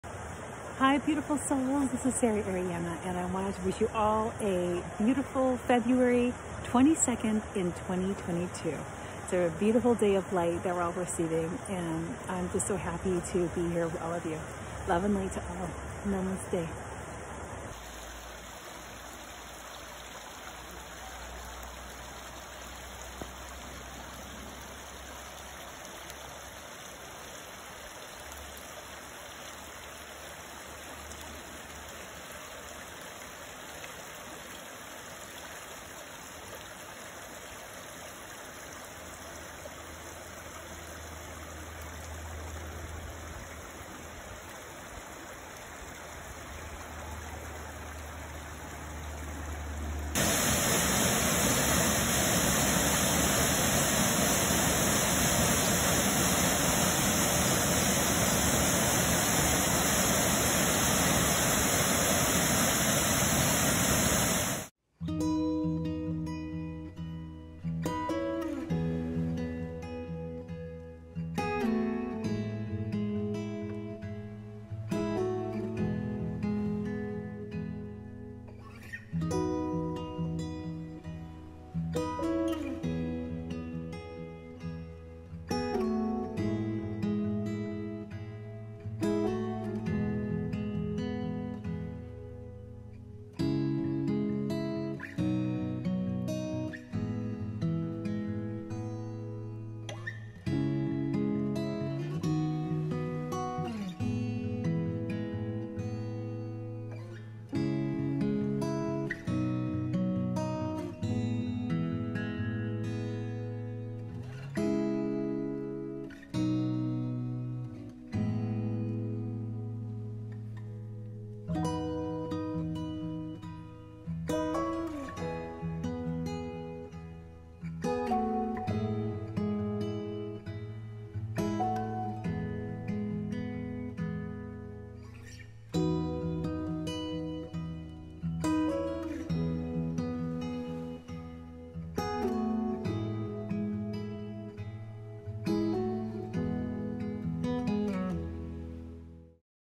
Happy 2•22•2022!!!
We are all receiving a beautiful, uplifting day of Light today!
This a powerful day to write down your intentions for what you would like in your life & our world.

Here is a video of fairy and waterfall energy!

I am so grateful to each and every one of you for being on the planet at this time.